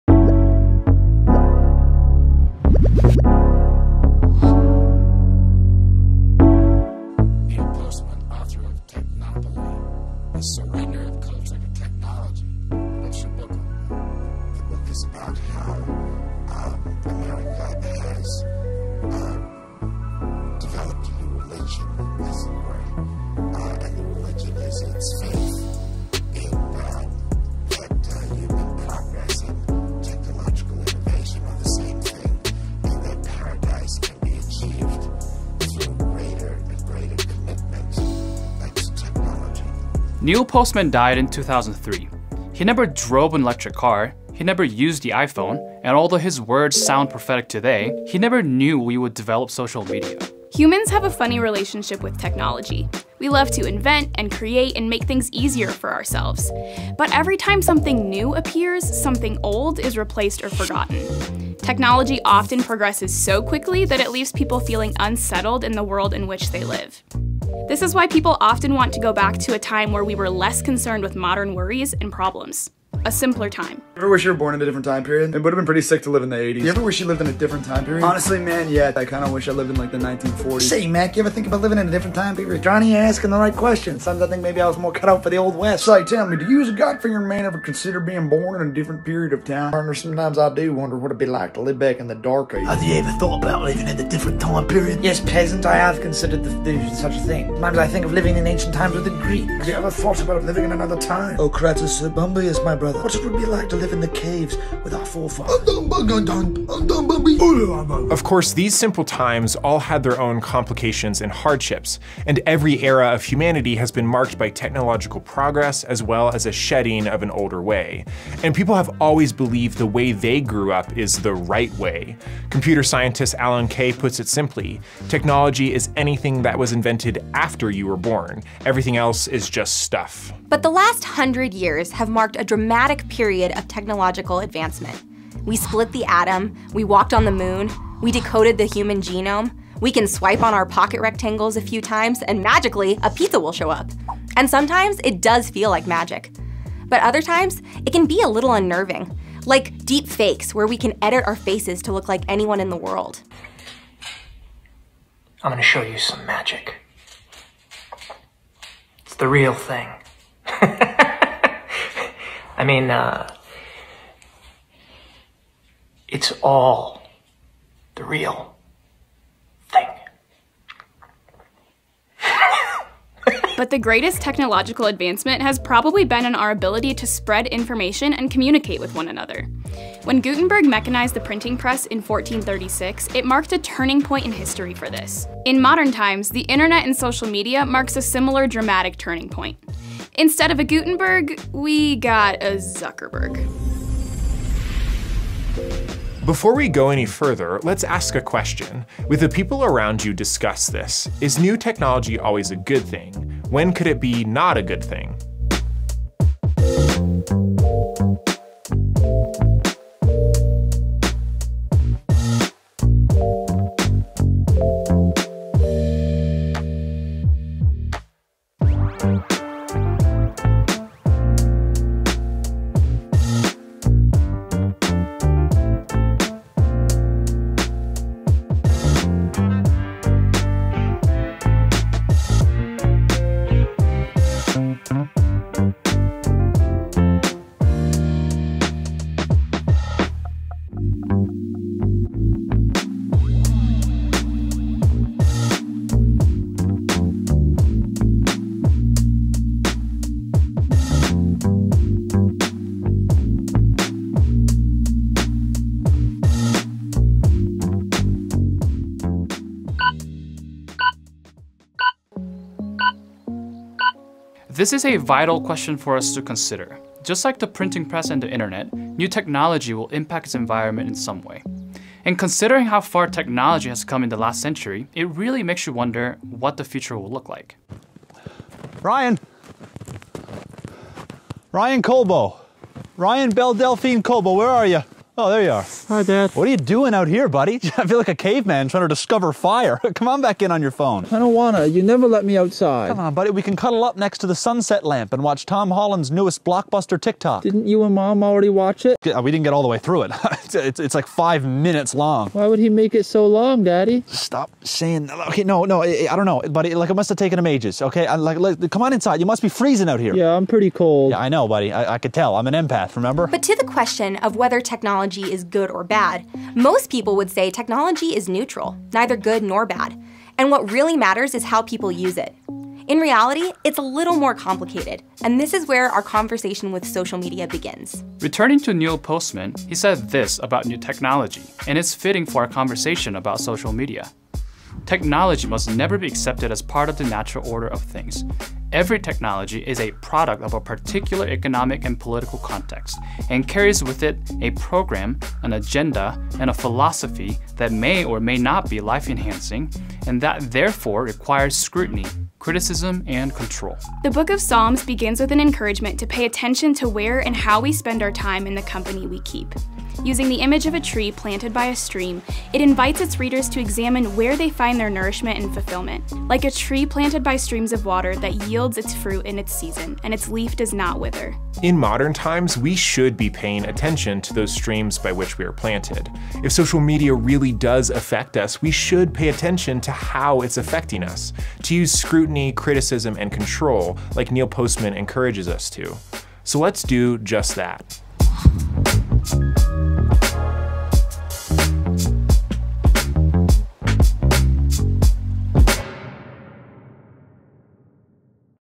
Social Media Episode 1: From One Berg to Another
Conversation Kits are a multi-episode video series meant to inform and start conversations between parents or caring adults and their teenagers.

Episode Summary:
   - We’ve gone from communication taking weeks to being practically instantaneous in the space of 150 years.
   - With any new technology, something old is set aside or lost to make space for the new thing. This can leave people feeling unsettled in the “new” world in which they live.
   - It’s important to pay attention to how technology influences the way we think and behave, as well as where we are looking for nourishment.

For more resources, go to axis.org